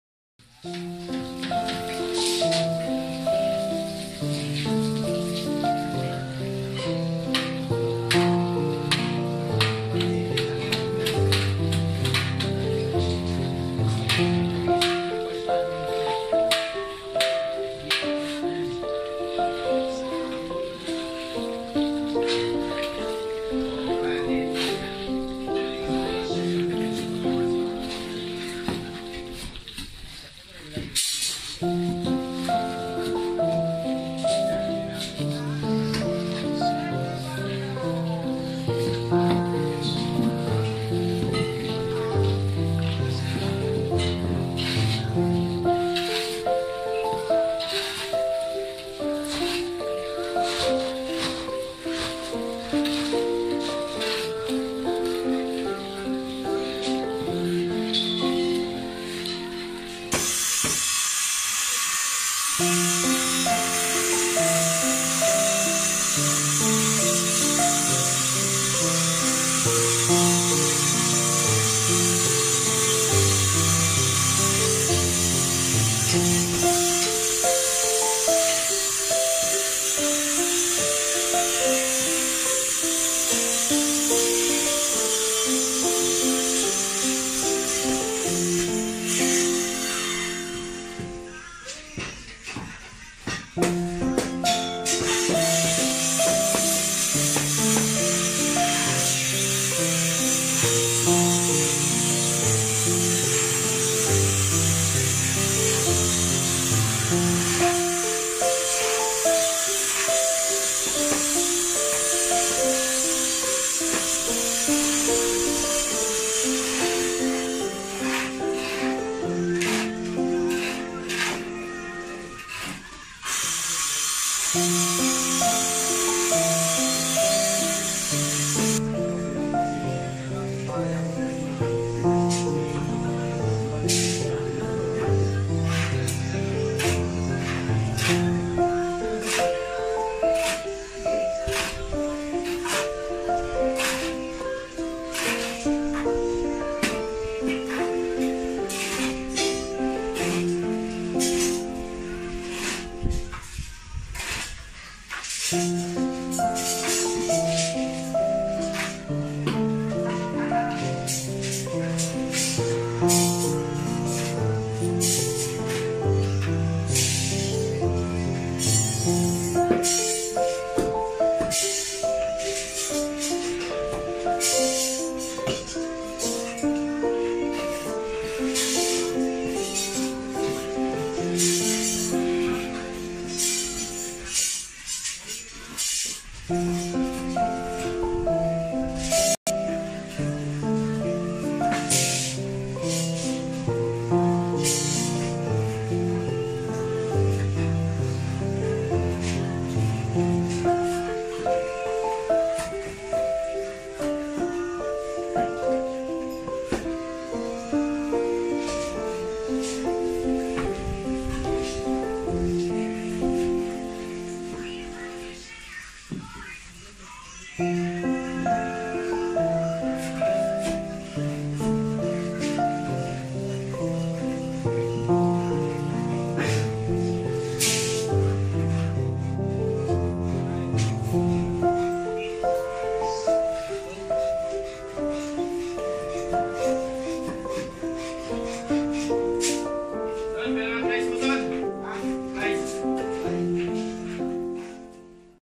Why is it stressful building a house in Philippines? Vlog #: 0003 | How to mix the cement and sand.
How to mix the cement and sand manually?
For mixing concrete is 1 cement : 2 sand : 3 gravel by volume. Mix the dry ingredients and slowly add water until the concrete is workable. This mixture may need to be modified depending on the aggregate used to provide a concrete of the right workability.